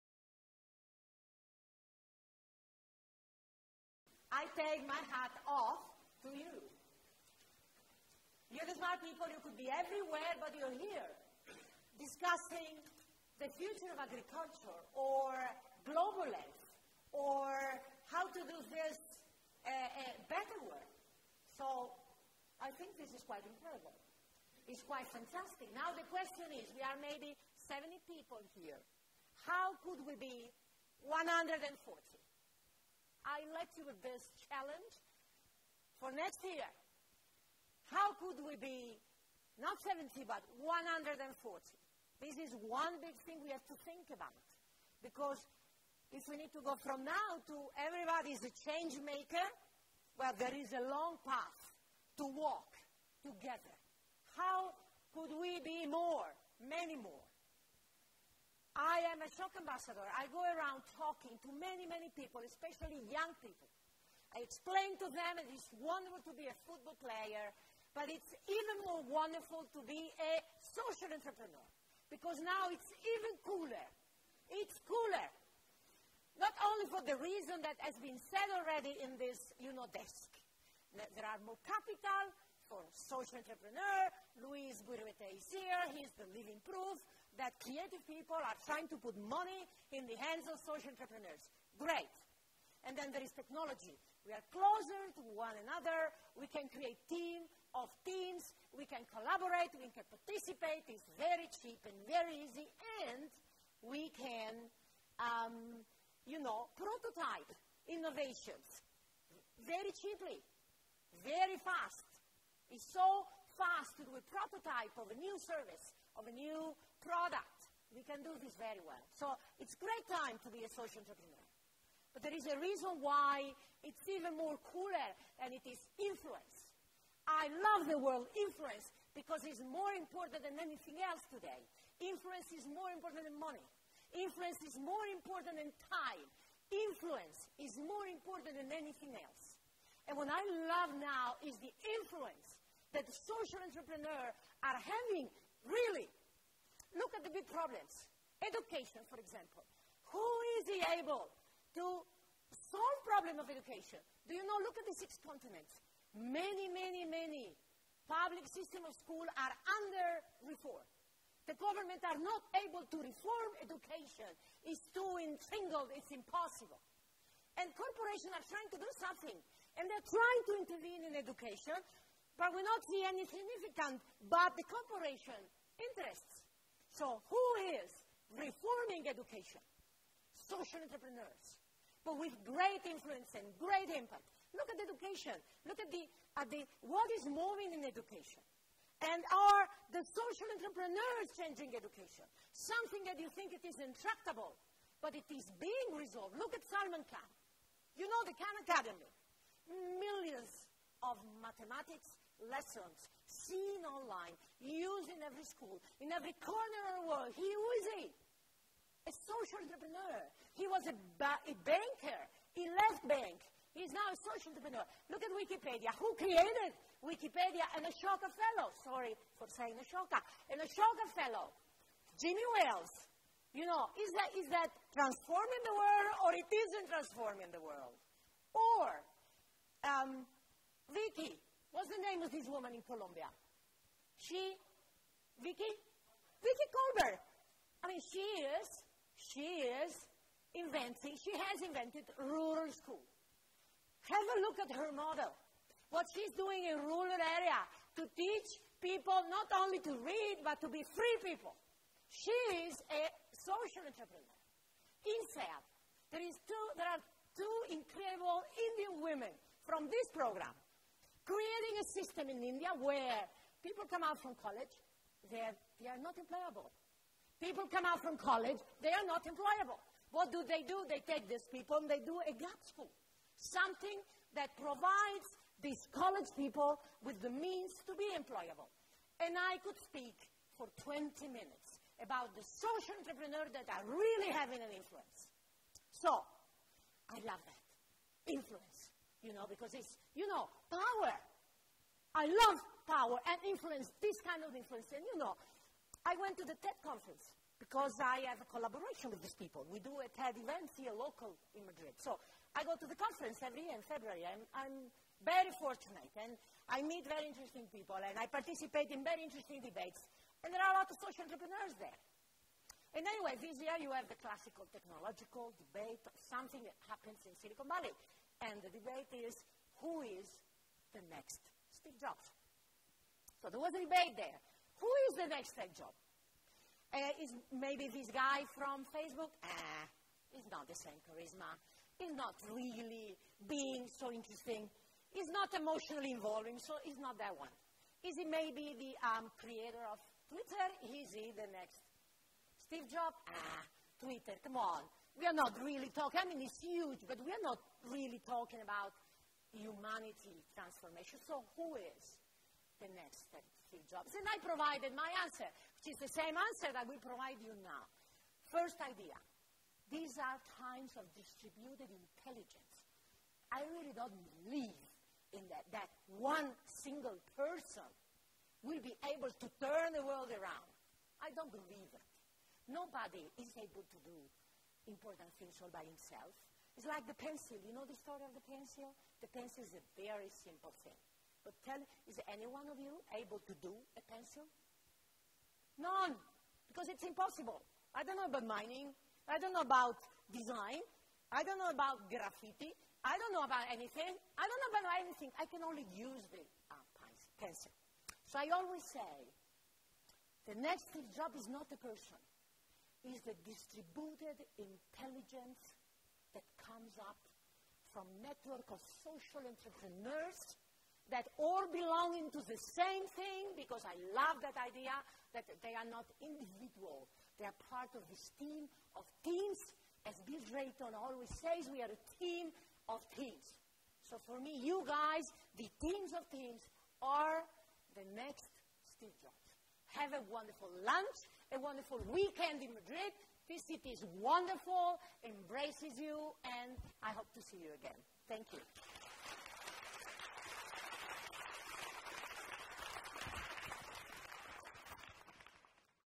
ANTONELLA BROGLIA (TEDx): Technology, Innovation and Social Change
INSEAD Social Entrepreneurship Conference
25-26 April 2013, Madrid
INSEAD, itdUPM & Fundación Banesto